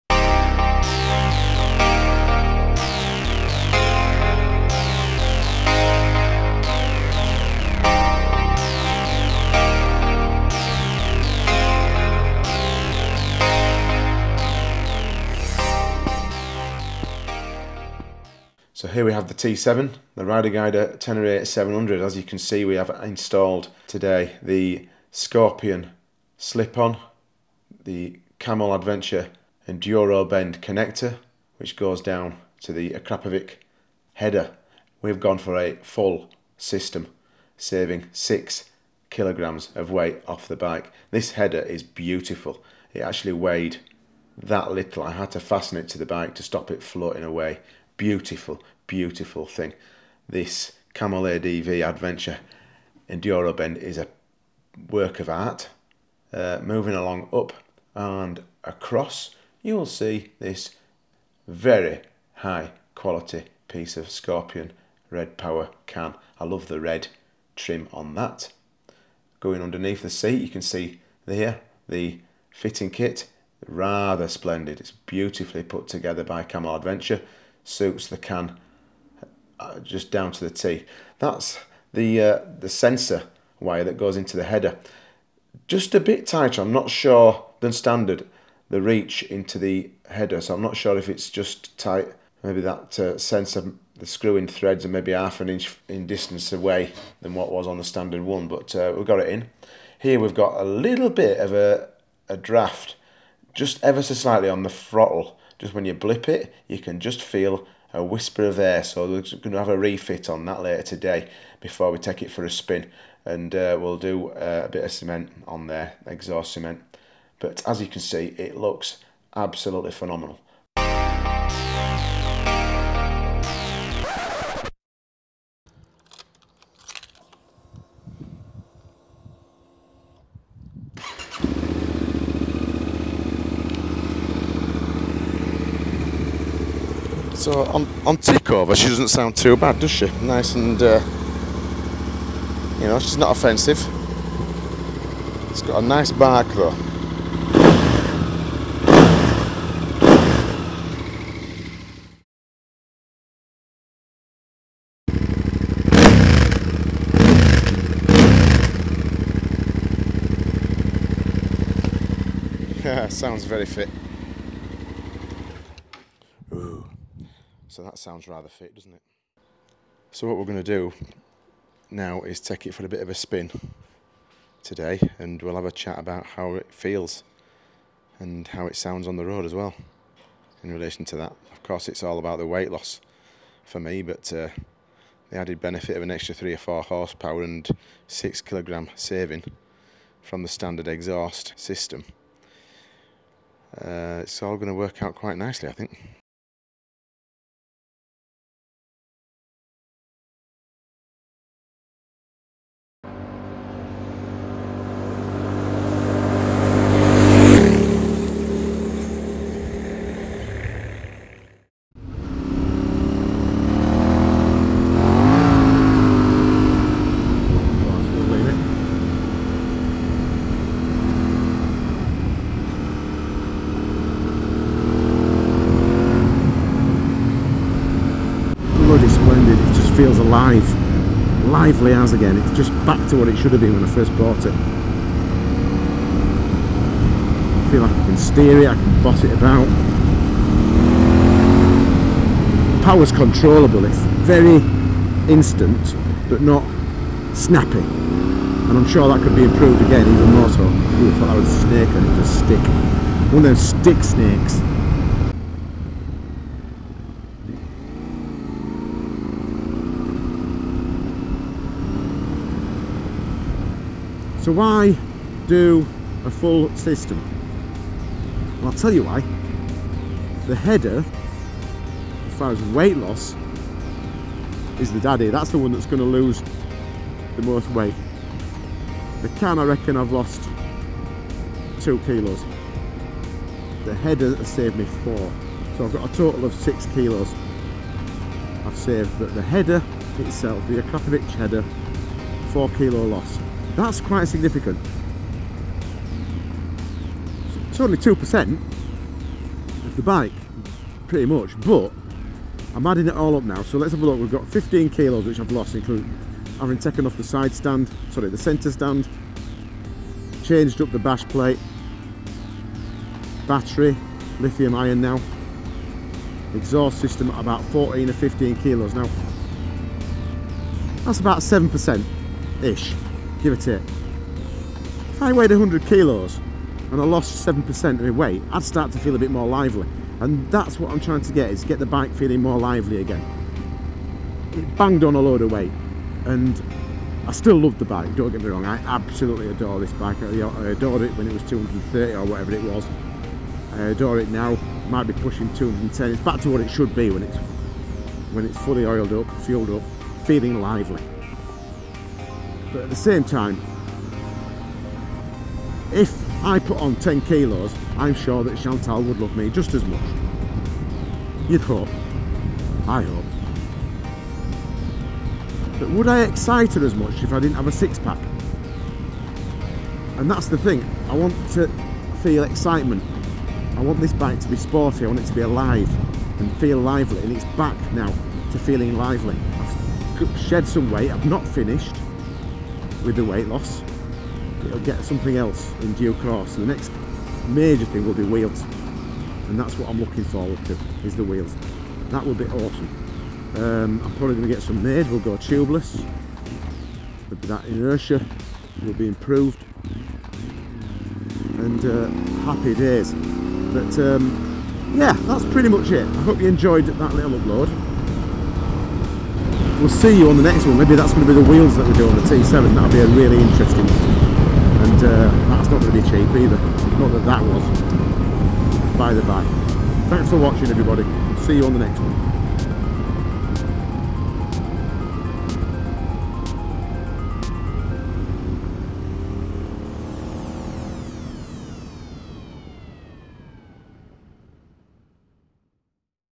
YAMAHA TENERE 700 FULL EXHAUST SYSTEM UPGRADE, SHOW, AND TEST.
In my quest to lower the weight of my Tenere 700 I've fitted a full exhaust system upgrade.
I've used the 2.1 kilo Akrapovic header, the Camel ADV enduro bend connector, and then finished it off with the beautiful Scorpion Red Power slip-on end can. These alone reducing the weight of my T7 by six kilos! In this video I'll show you around the bike, how it looks, of course how it sounds (as much as a video will allow), and of course maybe a little road test and 'fly-by' plus a quick chat on what's next for the Rider Guider T7.

This is not an install video, this was not that difficult to fit with some basic mechanic tools on hand, 60 - 120 mins however if you have any questions on fitting please ask or comment and I'll be more than happy to give my advice.